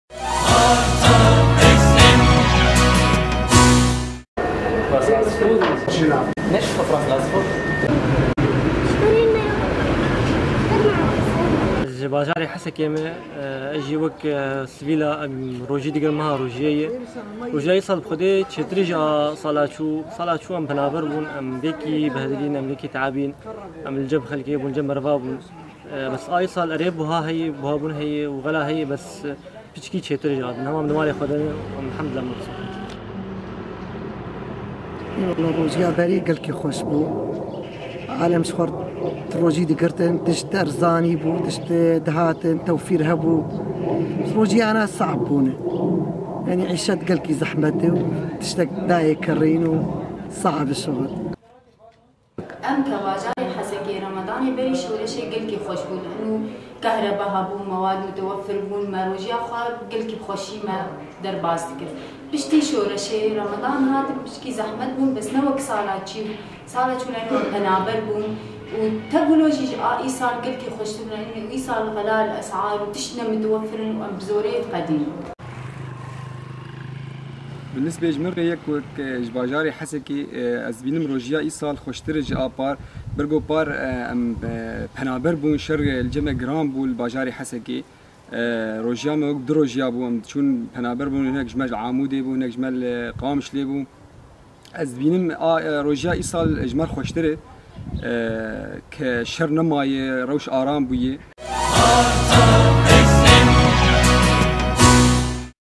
استطلاع حول أجواء شهر رمضان في الحسكة 19/06/2016
ما التغييرات التي حصلت في شهر رمضان بالنسبة لأهالي الحسكة، مقارنة بالسنة الماضية؟